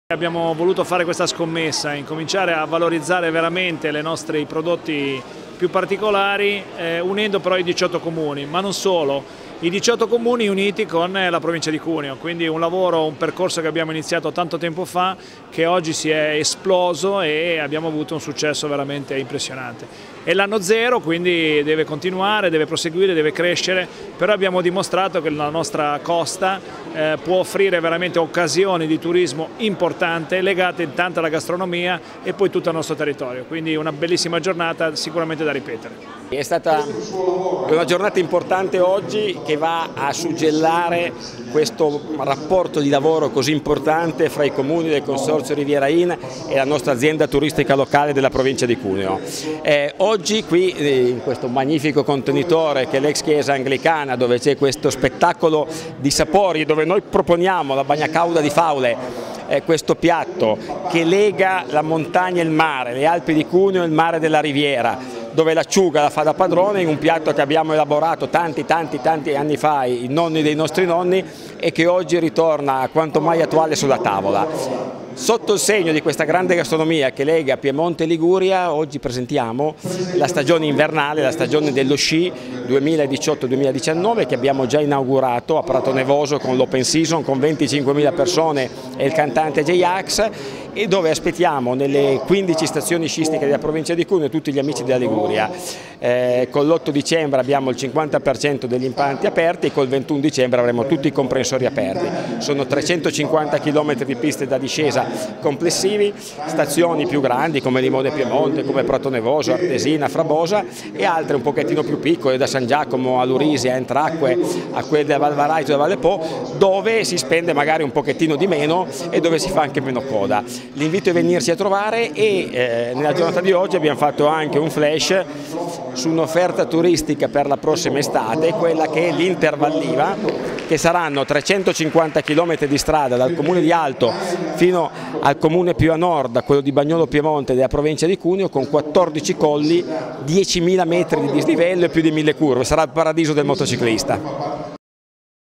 A BORDIGHERA NUOVO INCONTRO FRA LIGURIA E PIEMONTE
L’organizzazione turistica In Riviera, che raggruppa i Comuni del Ponente ligure e l’Atl di Cuneo hanno dato vita, a Bordighera, a un fine settimana all’insegna della cooperazione fra territori per la promozione turistica. Fulcro delle iniziative i prodotti tipici locali, come ci hanno spiegato il responsabile de In Riviera Sergio Scibilia e il direttore dell’Atl di Cuneo  Paolo Bongioanni.